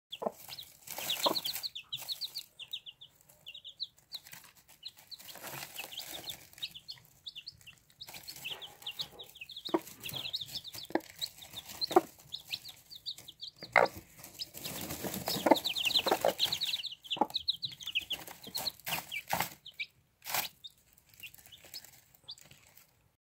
Hatched baby chicks eating with mother hen.
Hatched baby chicks eating with mother hen. Miss Caramel and her babies in the morning .